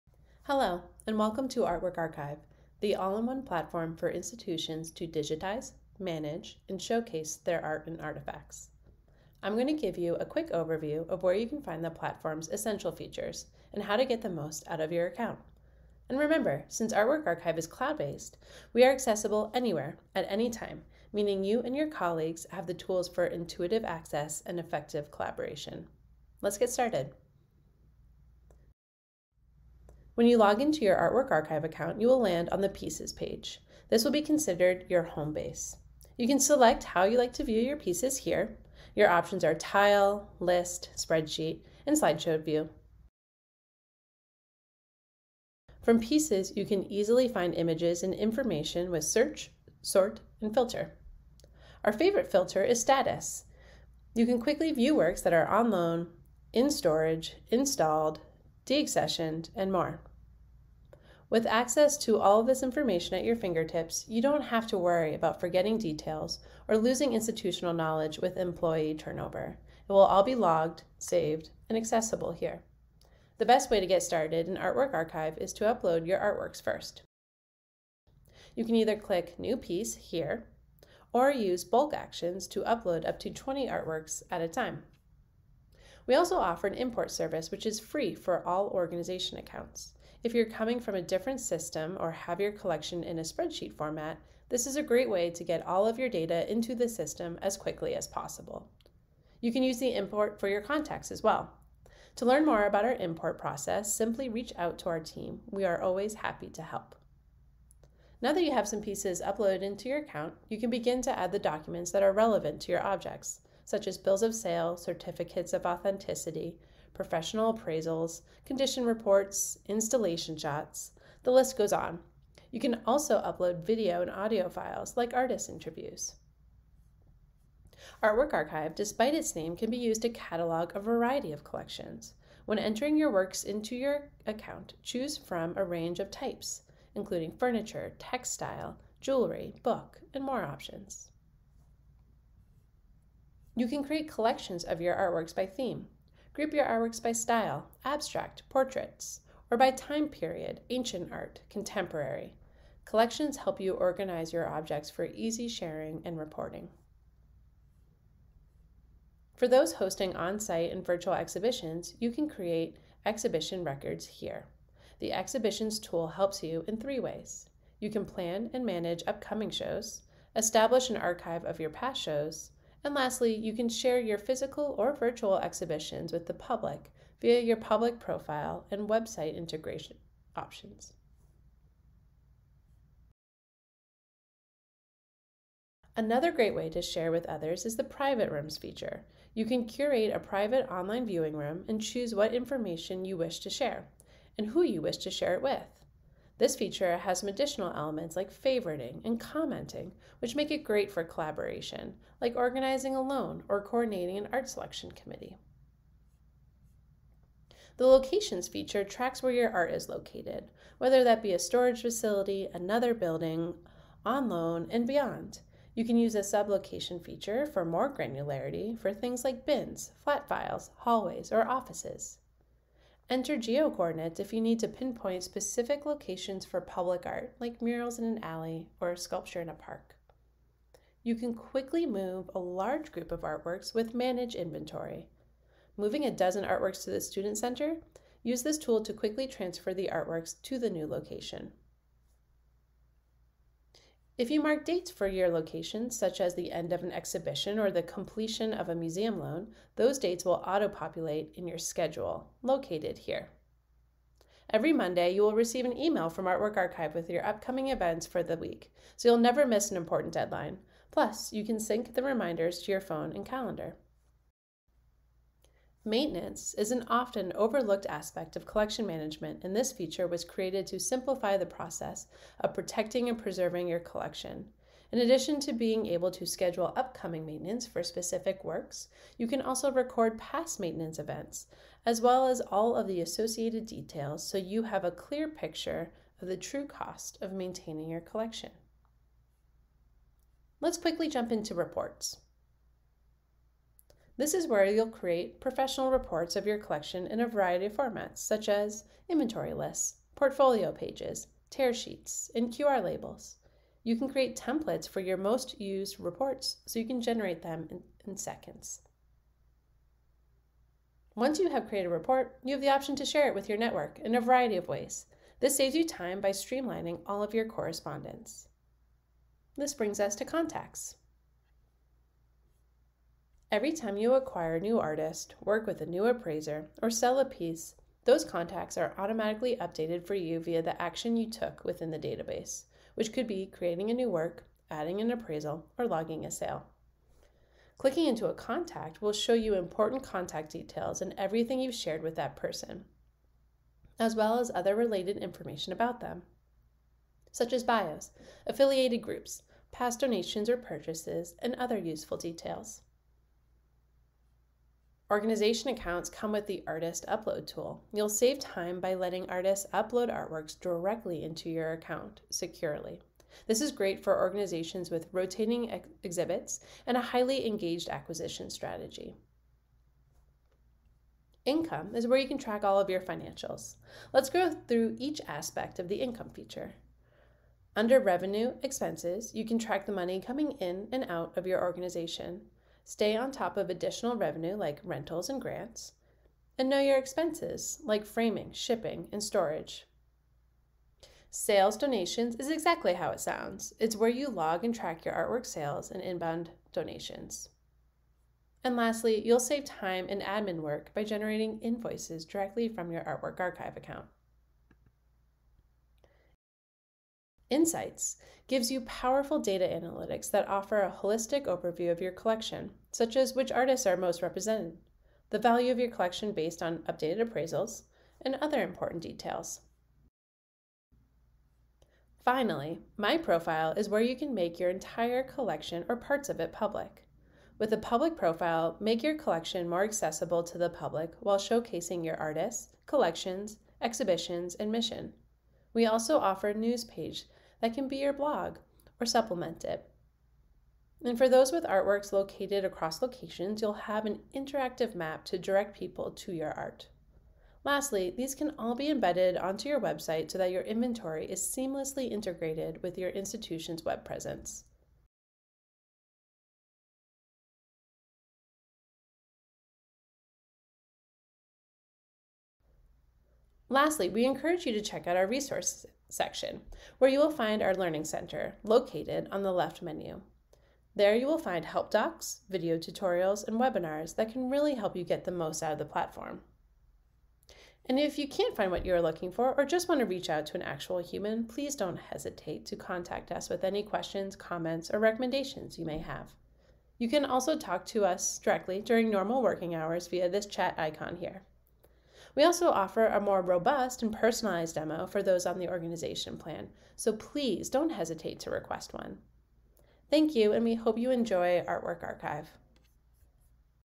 Getting Started with Artwork Archive for Organizations
This video gives an introduction into the Artwork Archive platform for Organizations.